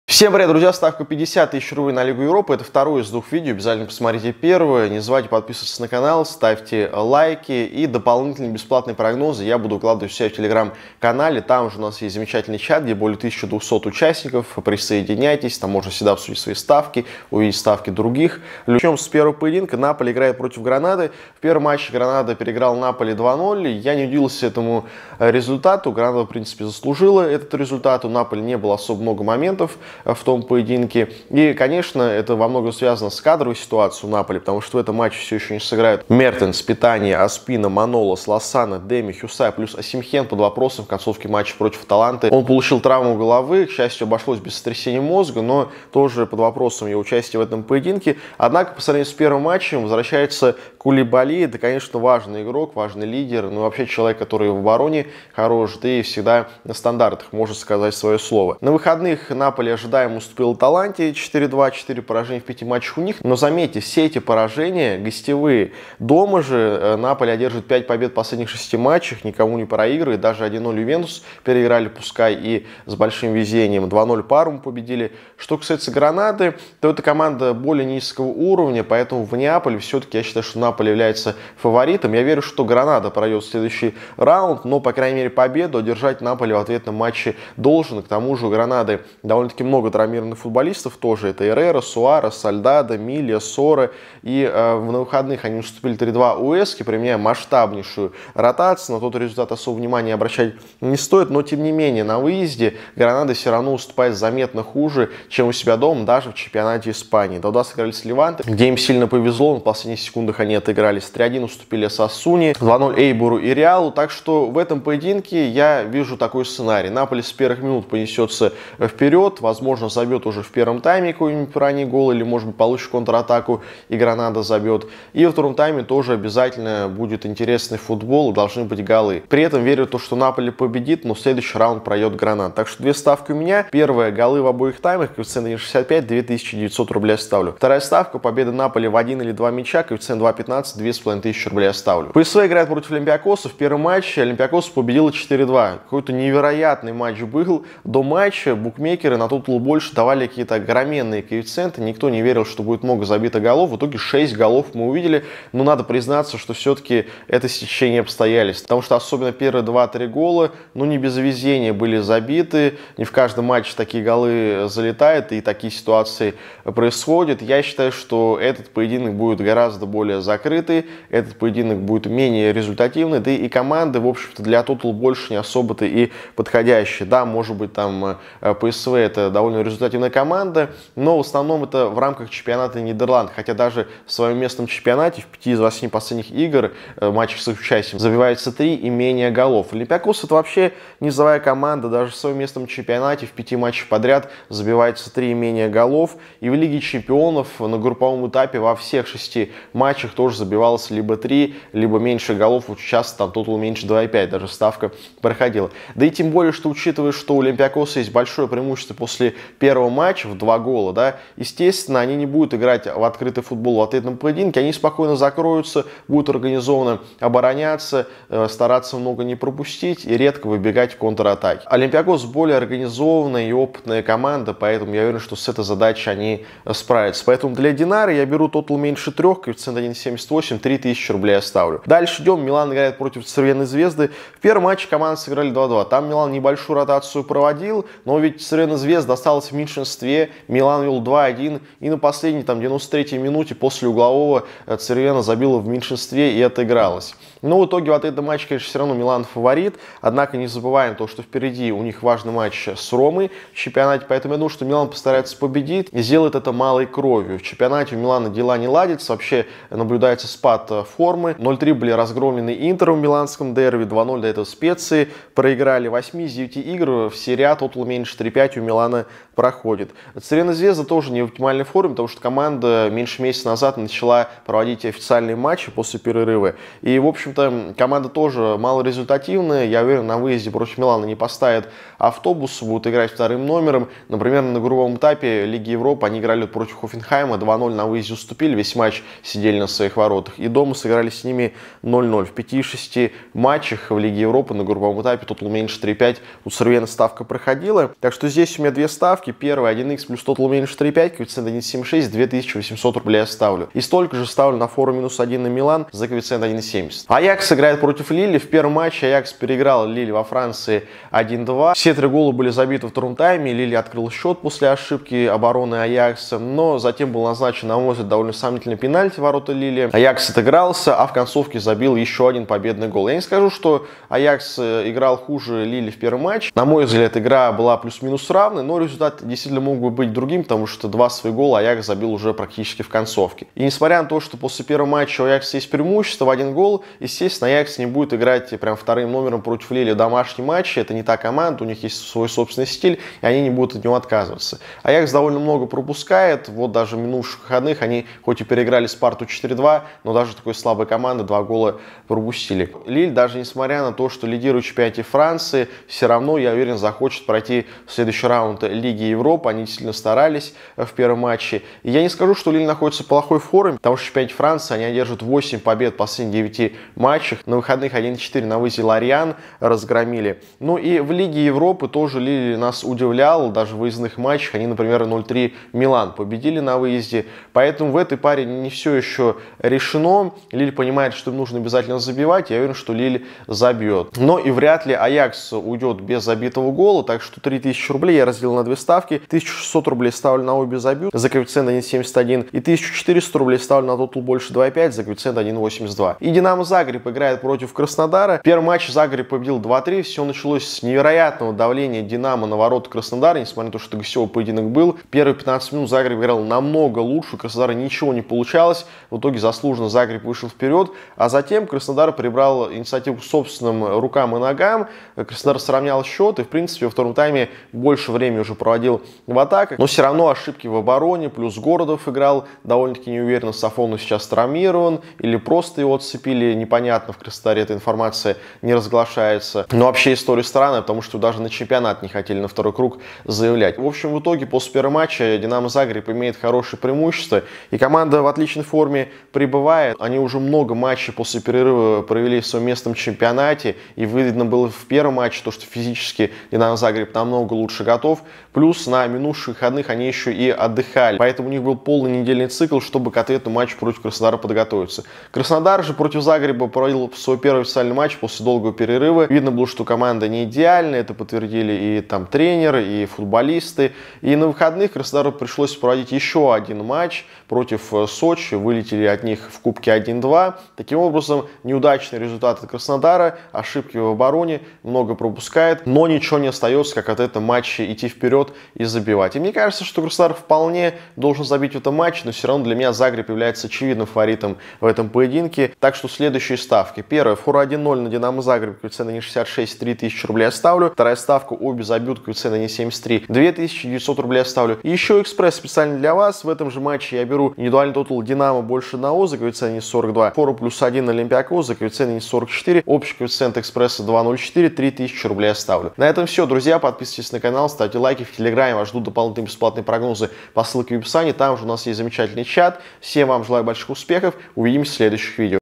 СТАВКА 50 000 РУБЛЕЙ! ДИНАМО ЗАГРЕБ - КРАСНОДАР. АЯКС - ЛИЛЛЬ. НАПОЛИ - ГРАНАДА ПРОГНОЗ ЛИГА ЕВРОПЫ
Экспресс Олега Соловьева на Лигу Европы!
Ставка 50 000 рублей на футбол! Голы и Обзор матчей.
Прогноз и бесплатная ставка от Олега Соловьева на матчи матчи Лиги Европы Динамо Загреб - Краснодар / Аякс - Лилль / Наполи - Гранада / Милан - Црвена Звезда / ПСВ - Олимпиакос.
Какая команда пройдет в 1/8 финала Лиги Европы?